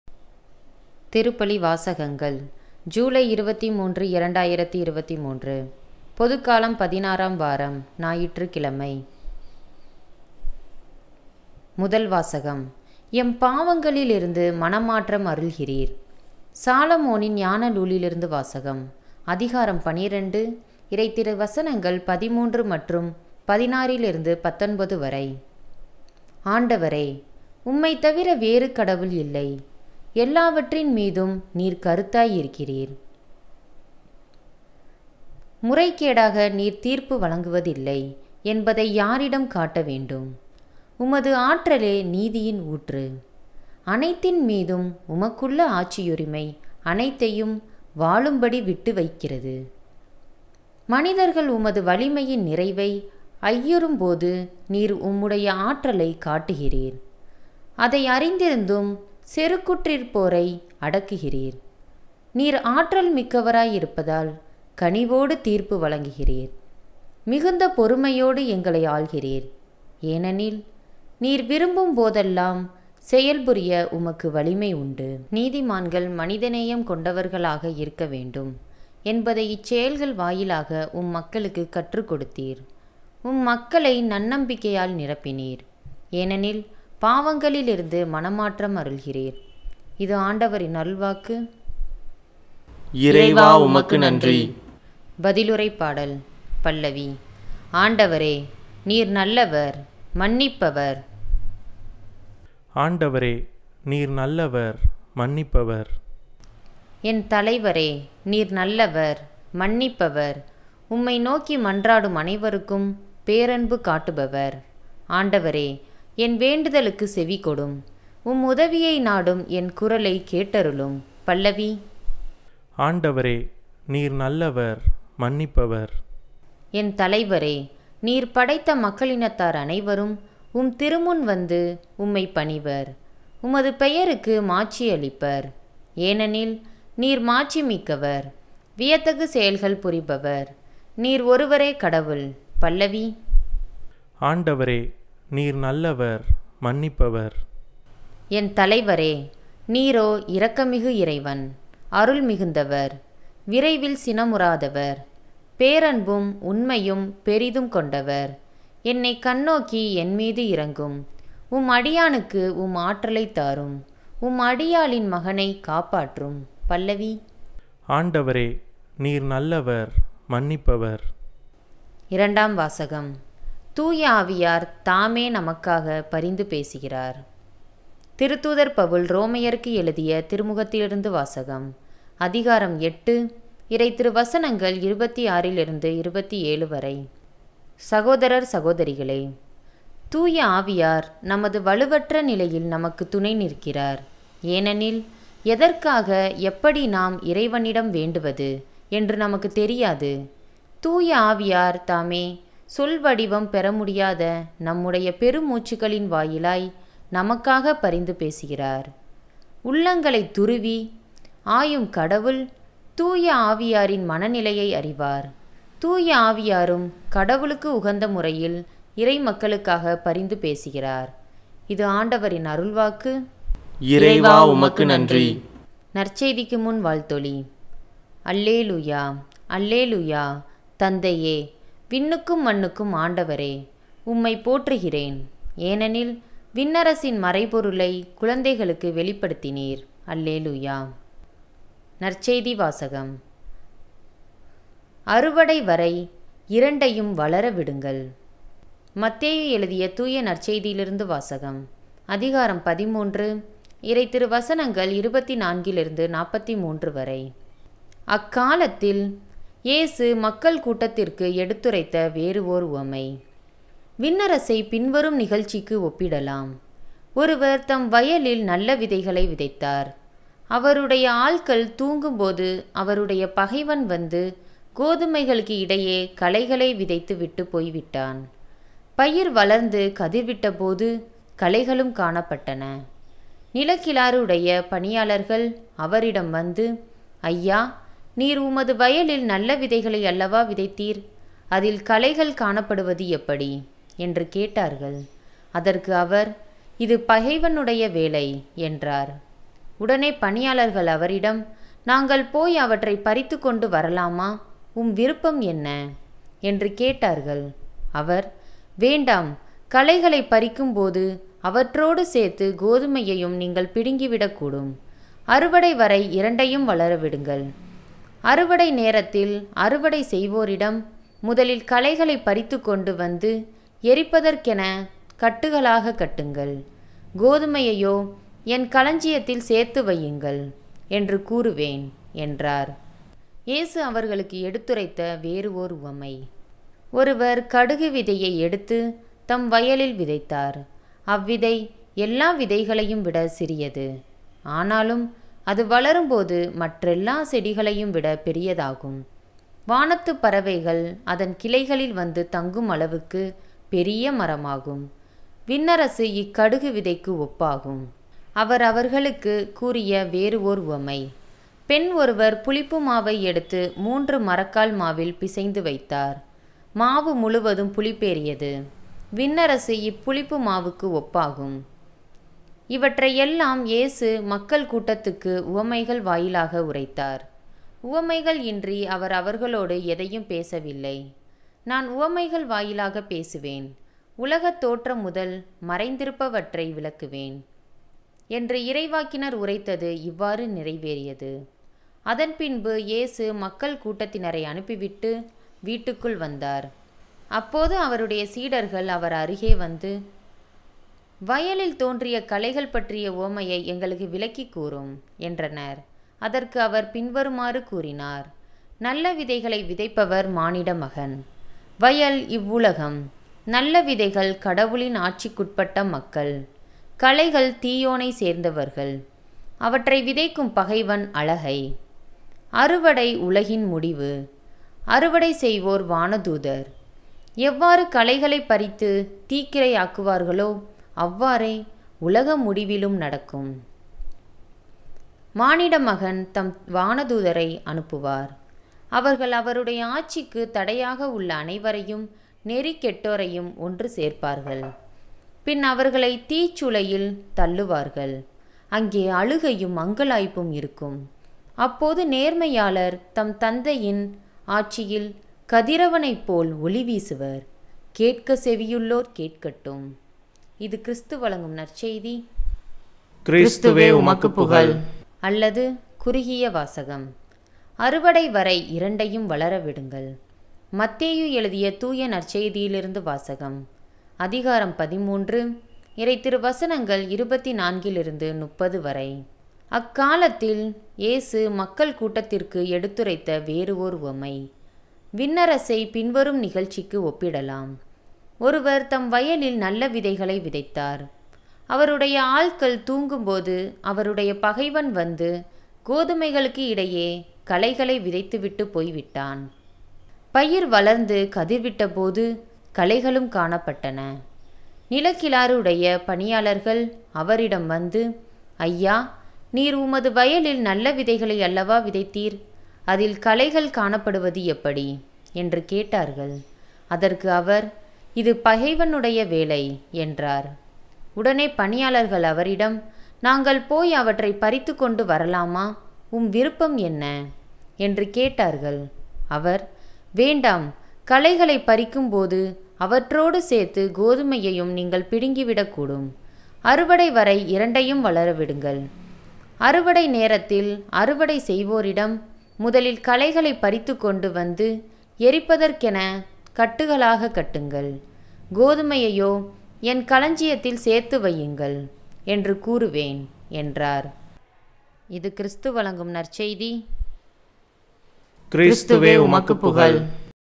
Mass Readings in Tamil - July 23 2023
திருப்பலி வாசகங்கள் – ஜூலை 23, 2023, பொதுக்காலம் 16ஆம் வாரம் – ஞாயிறு.